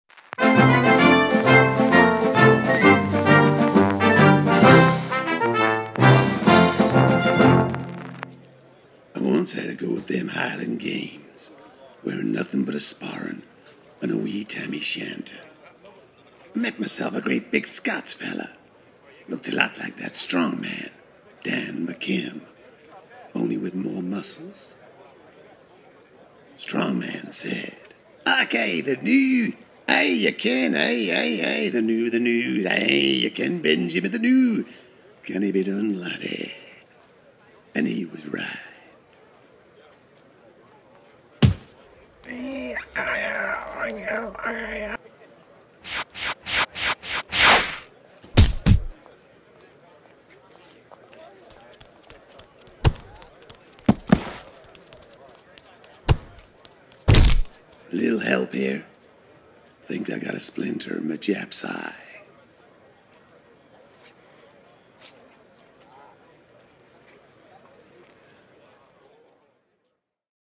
TOSSING the CABER HIGHLAND style
TOSSING the CABER HIGHLAND style. Captain Blunder takes part in the Highland Games wearing nothing but a wee sporran and a wee tam o'shanter. How will he perform. Watch this short, animated film to find out.